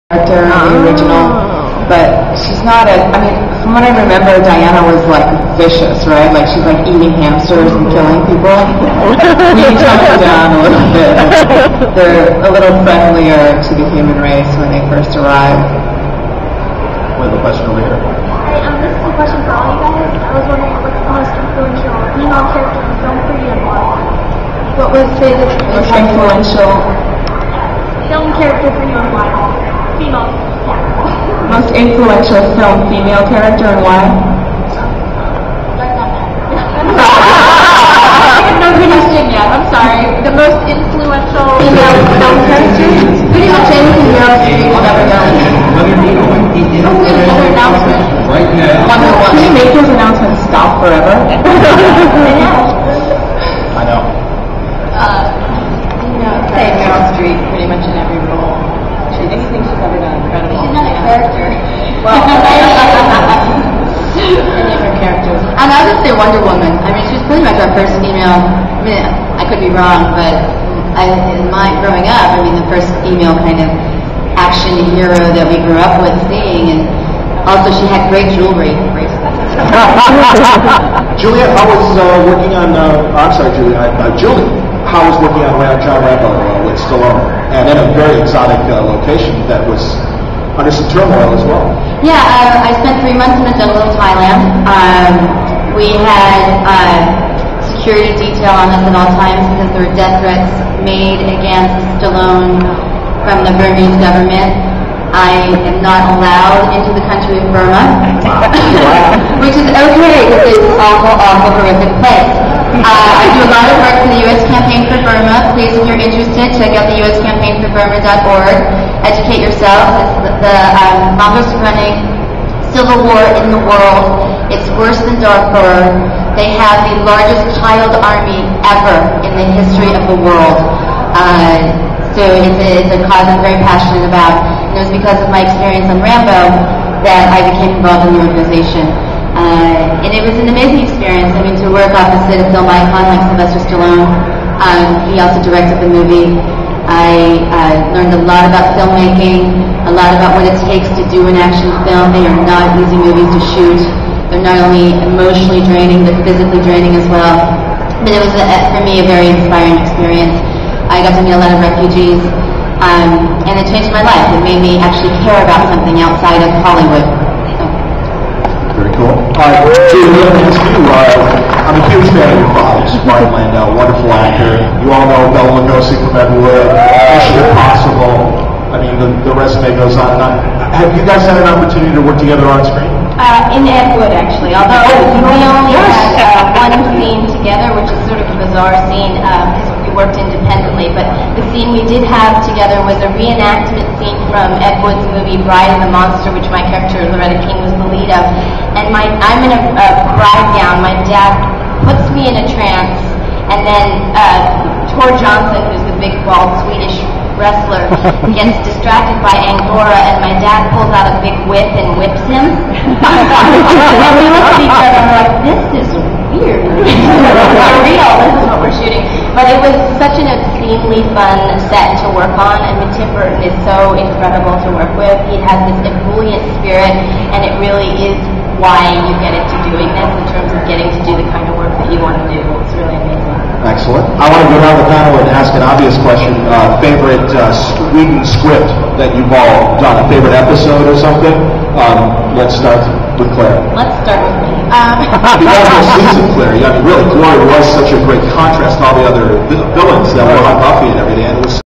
The Women of Whedonverse part 3
Julie Benz, Juliet Landau, Jewel Staite, Morena Baccarin and Clare Kramer at FX Show 2009 in Orlando, FL